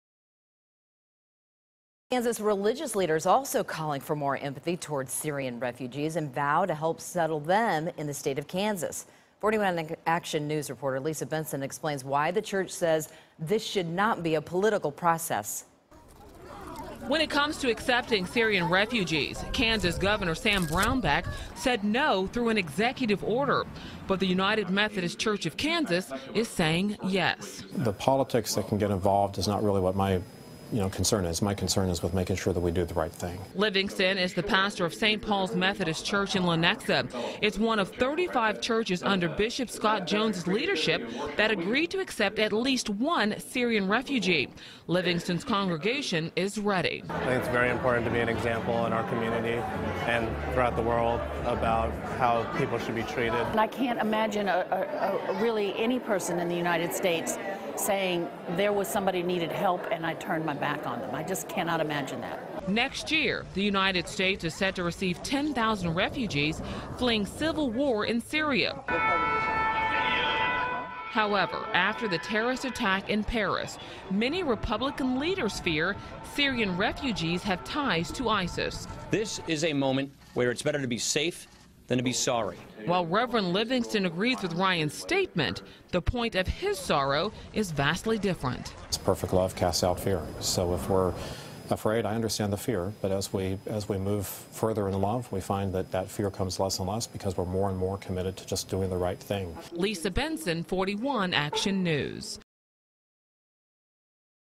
United Methodist Church of Kansas welcome Syrian refugees
Kansas religious leaders are calling for more empathy towards Syrian refugees and vow to help refugees resettle in the state.

41 Action News, KSHB, brings you the latest news, weather and investigative reports from both sides of the state line.